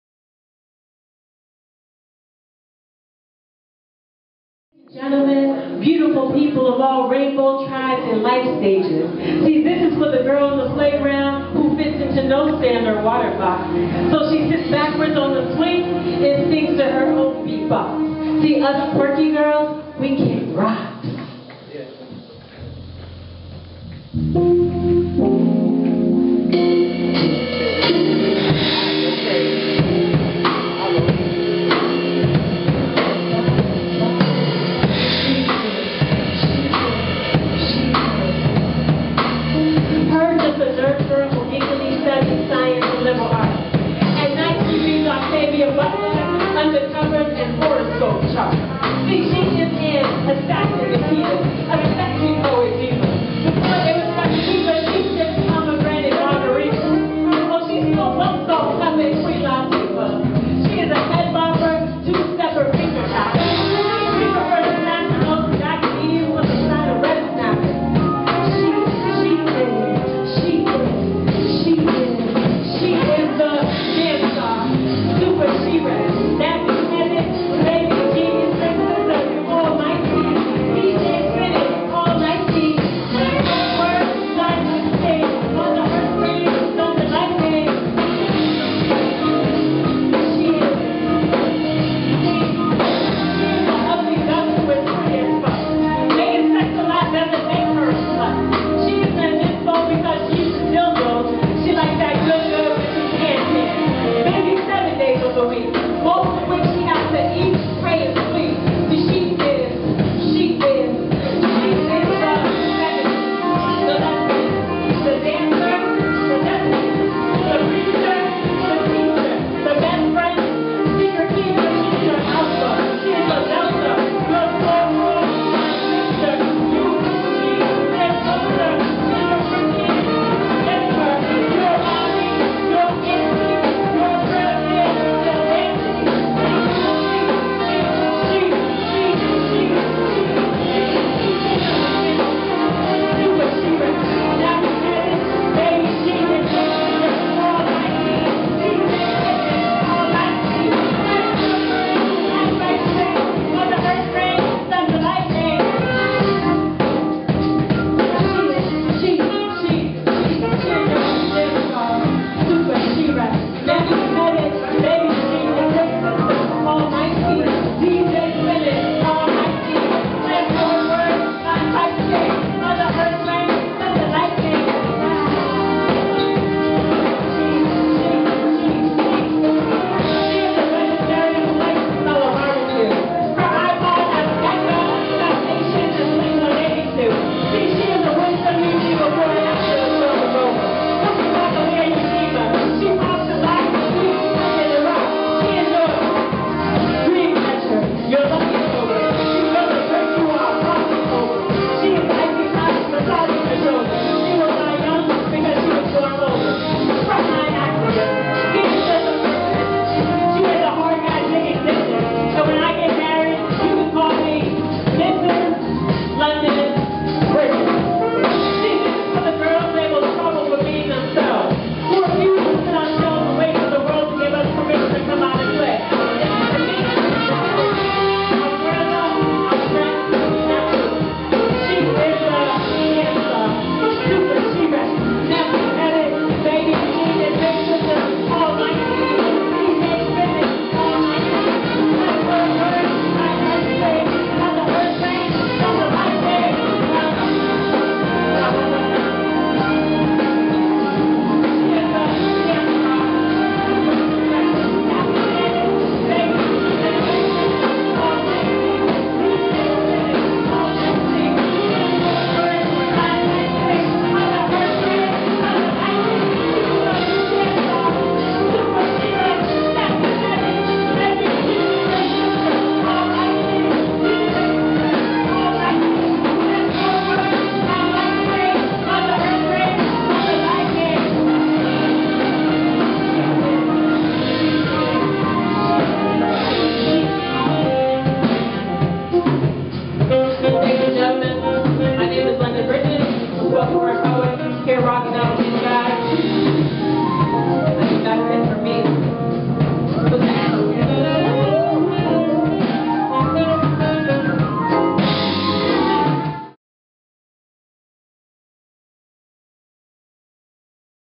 Jamara Mychelle Wakefield Spoken Word Poet + LIVE Band
Jamara Mychelle "London" Wakefield is an American spoken word poet, community organizer and writer, previously known by her stage name London Bridgez.[1][2][3][4][5][6]